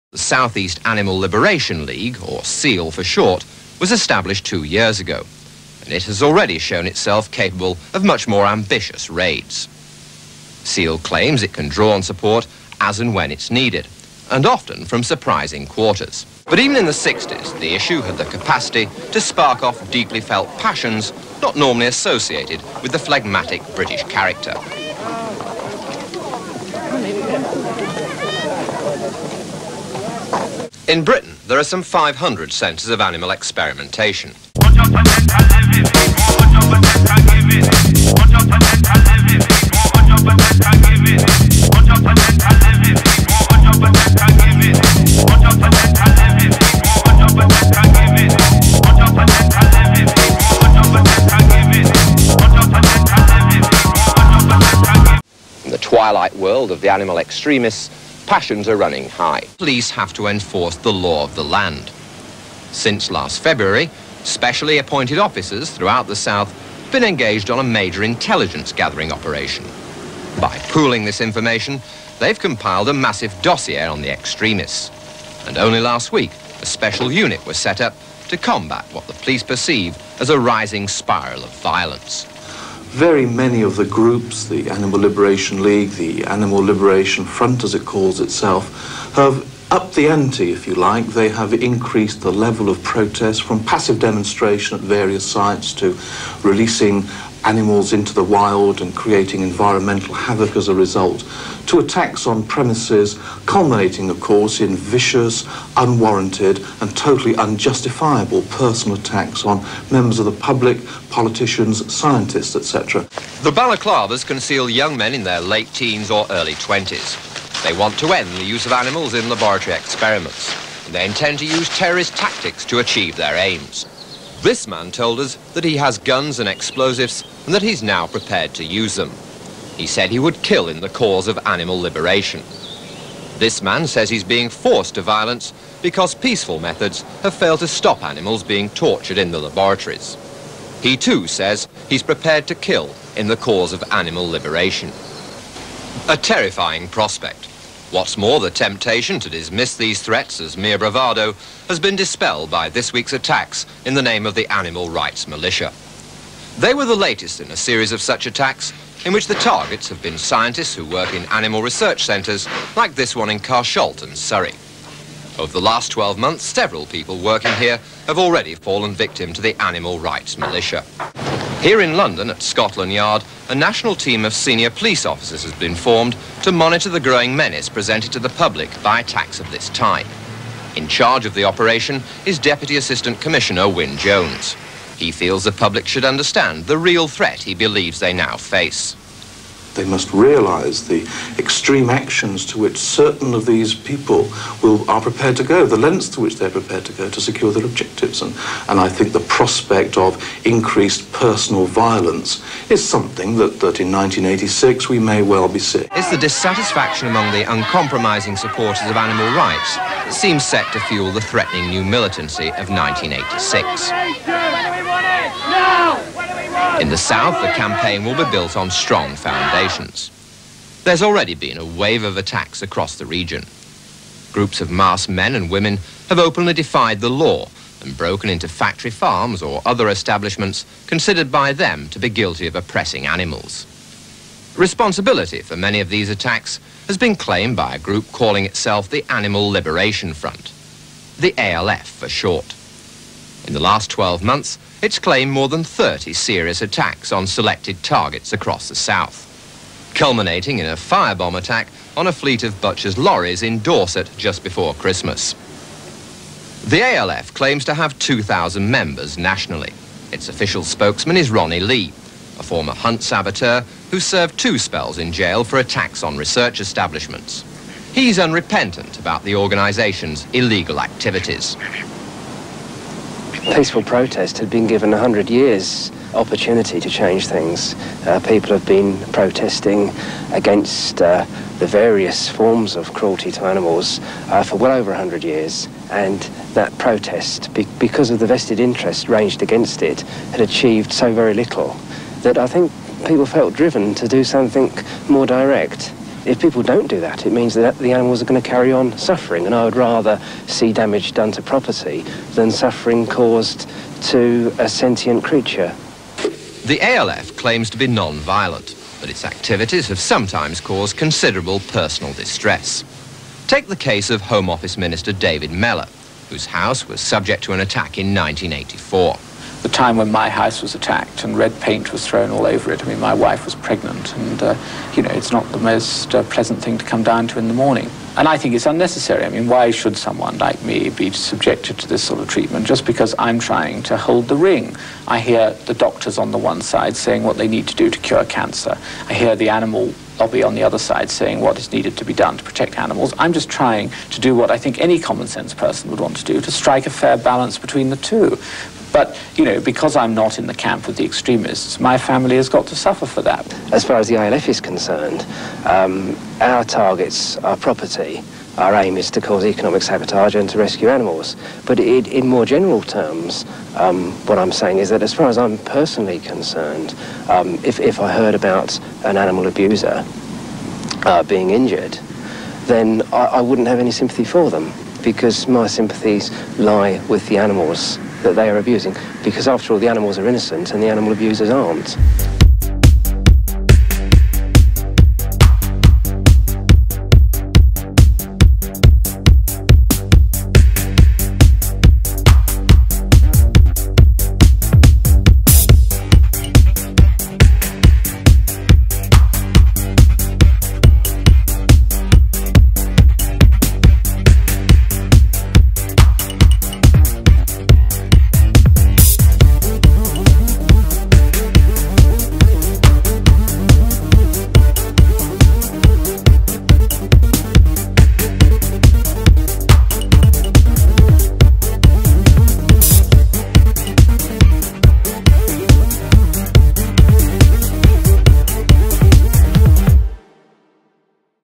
20,000 channel views!! (A short film on the 1986 ALF/ARM/SEALL and more..)
A video to say thanks for all the support, views and subscriptions since ALTV began.
Passing 20,000 views is a milestone for ALTV.
In this video are featured; news items from 1986, SEALL, Ronnie Lee (ALF Founder), ARNI, ALF/ARM and ALF Spain 2003 and more.

ALTV is a channel telling the history of direct action, it does not promote or encourage illegal behaviour.